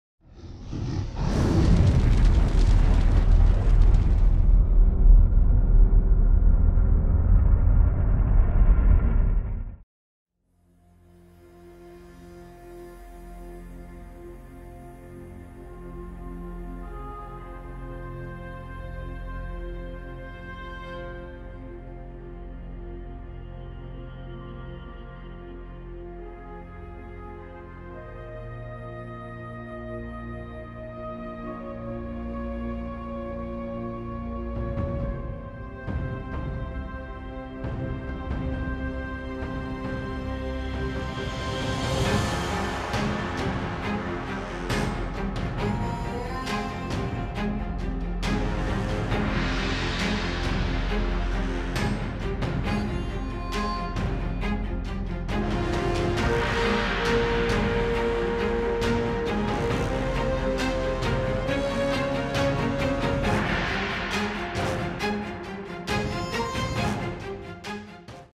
Quicksilver LED Dragon Statue by Ruth Thompson - Medieval Collectibles
The Quicksilver LED Dragon Statue by Ruth Thompson is painted by hand with exquisite attention to detail. This light-up dragon statue is crafted from cold cast resin, and its LED light shines from within a crystalline cavern.